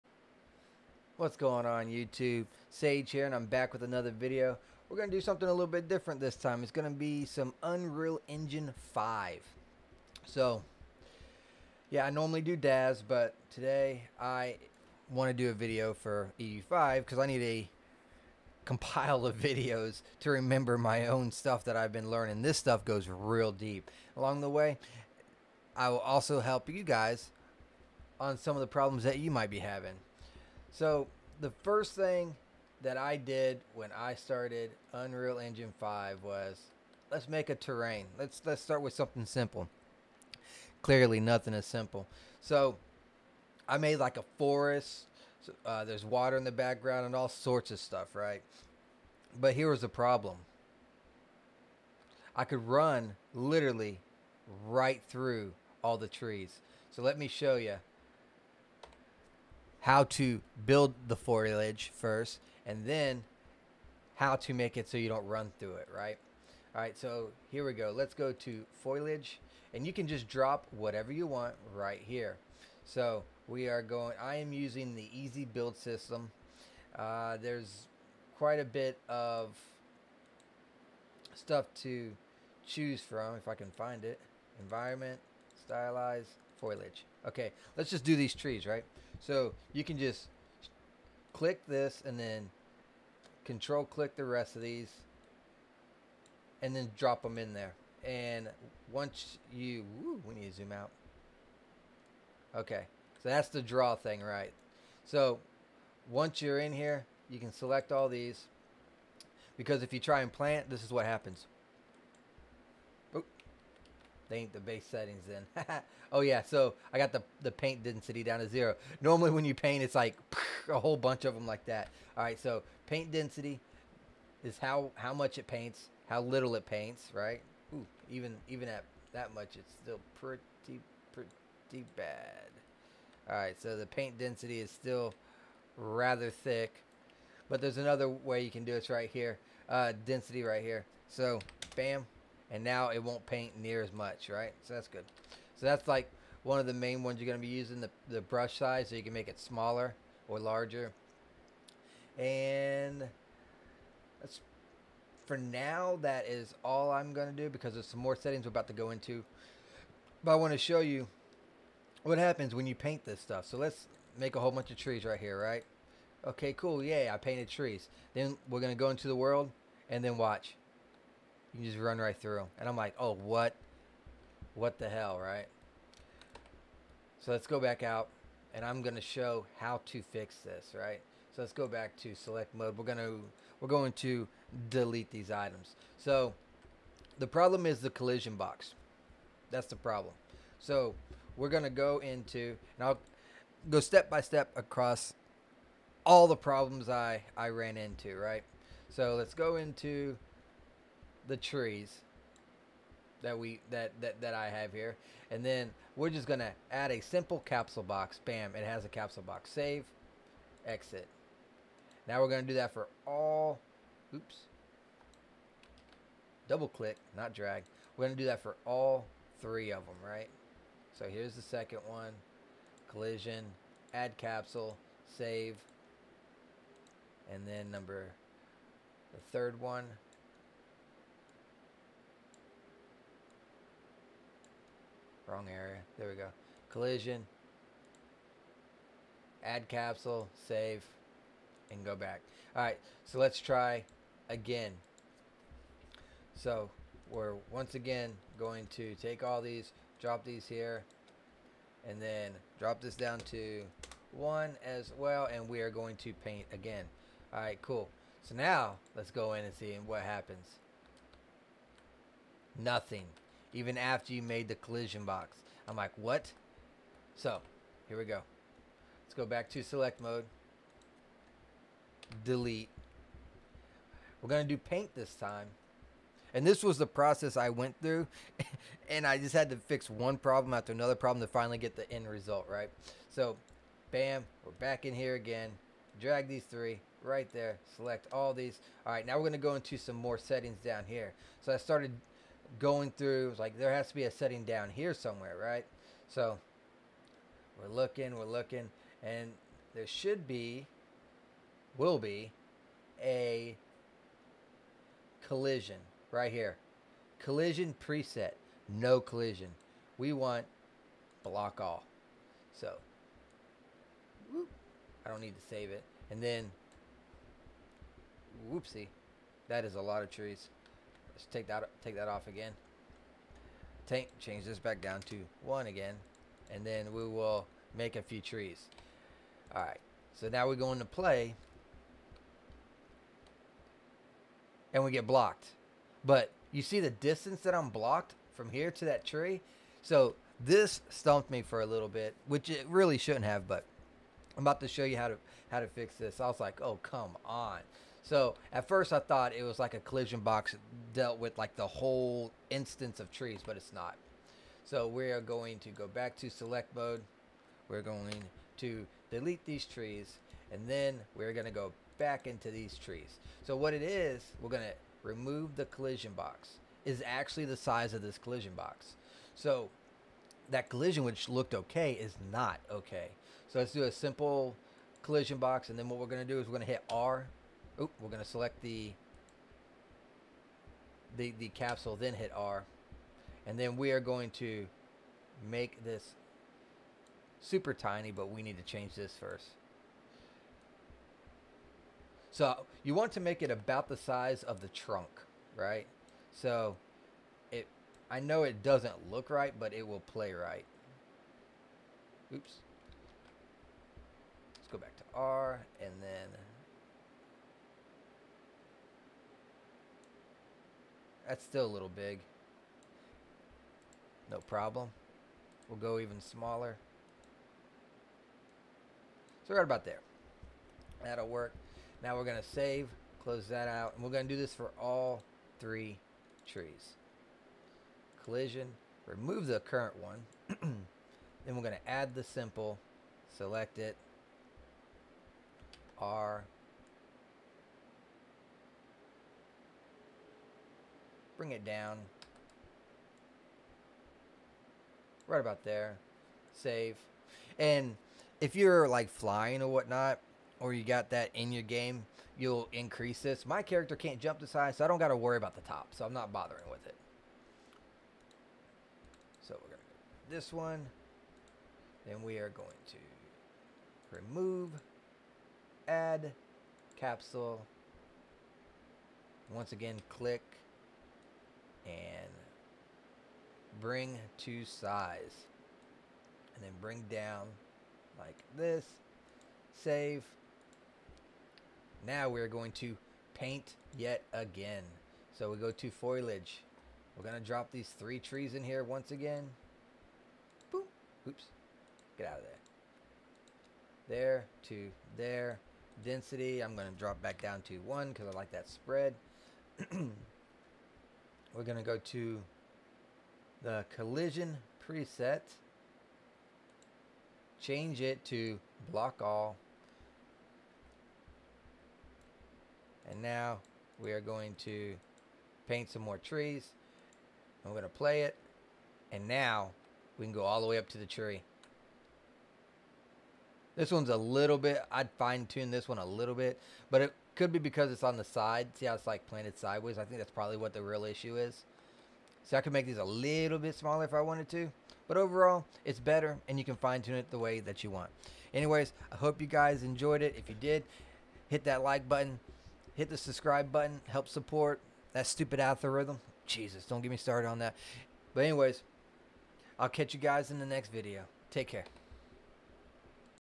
Creating foliage and fixing collision with objects
In this video I explain how I figured out the foliage collision. I how this helps you guy :D